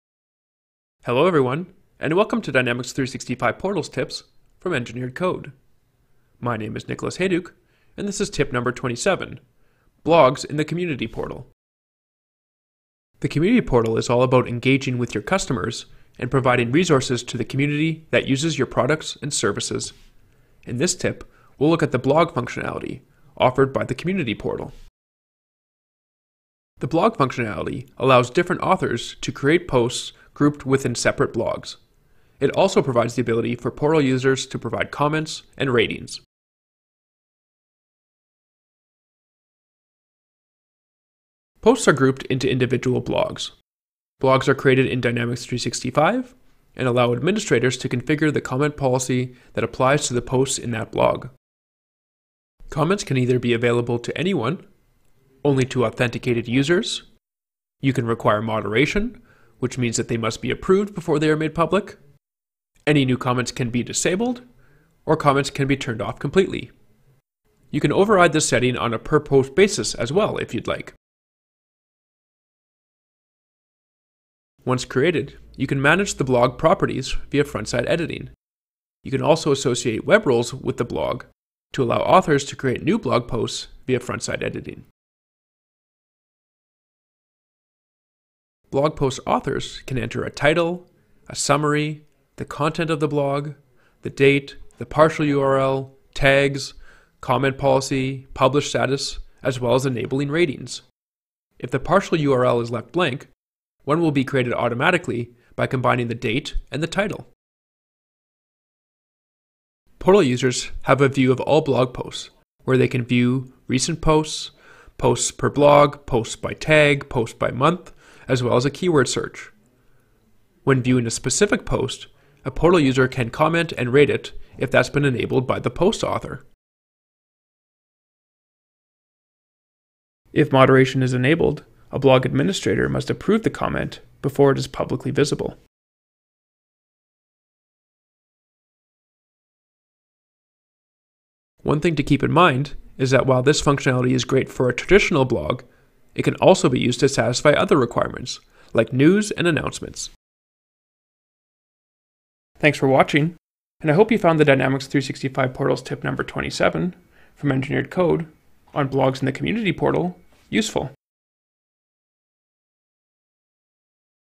Dynamics 365 Portals Tip #27 - Blogs in the Community Portal - Engineered Code
In this tip we dive into the Blog functionality offered as part of the Community Portal.  The blog functionality allows different authors to create posts grouped within separate blogs.  It also provides the ability for portal users to provide comments ratings.